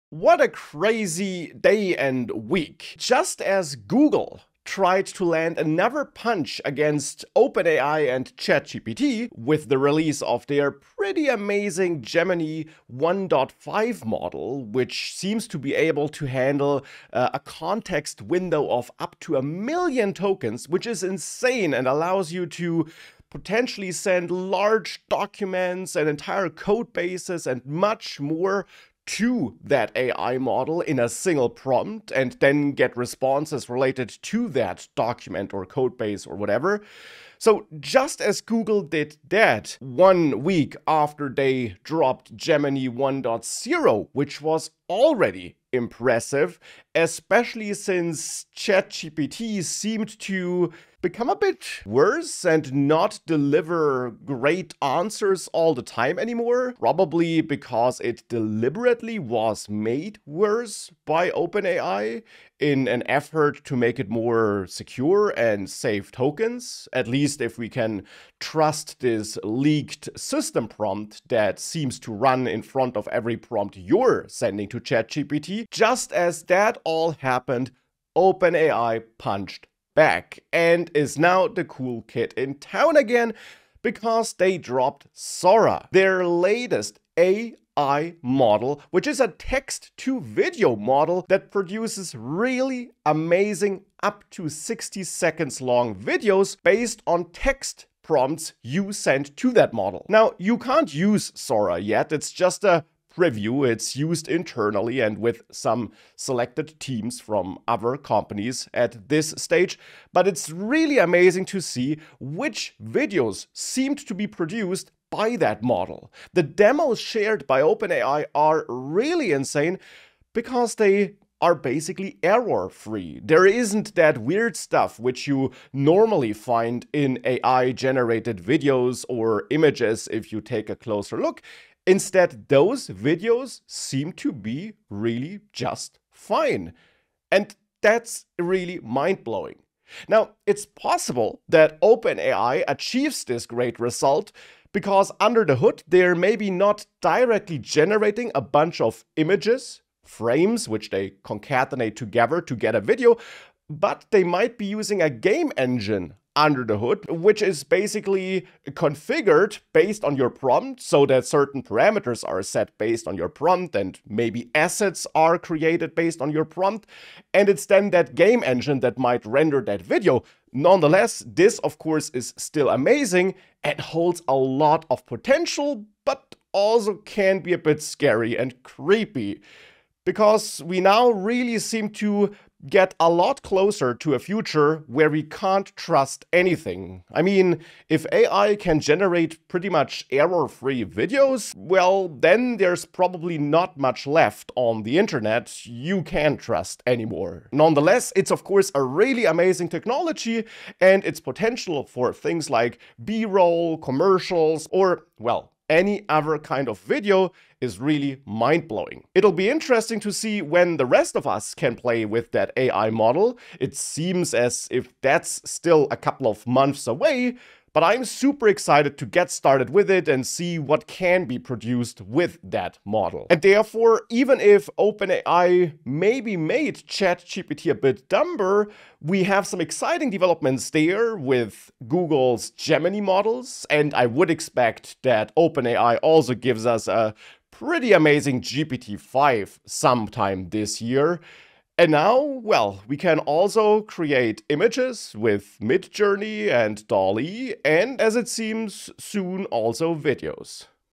OpenAI did it again ... with SORA!
Sora is OpenAI'S text-to-video AI and it's simply crazy...